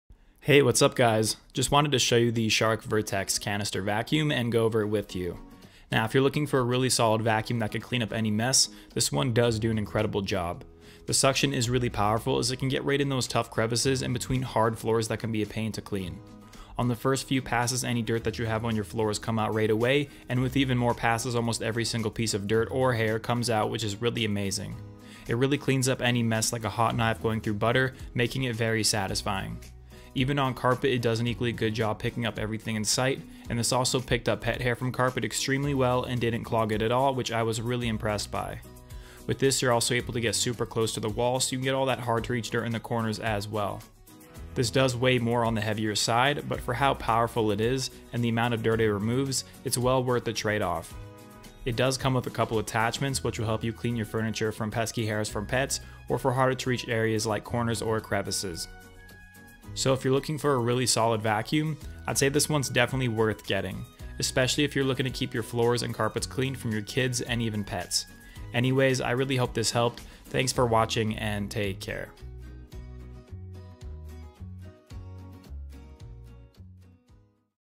The most powerful vacuum ever ll By BTM Store
Product Details,
Shark CZ351 Pet Canister Vacuum, Bagless, Corded with Self-Cleaning Brushroll & PowerFins, Navy & Silver

Brand: Shark
Special Feature: Corded Canister, Bagless, HEPA
Filter Type: Hepa, Foam, Felt
Surface Recommendation Carpet, Hard Floor & Upholstery

Thanks!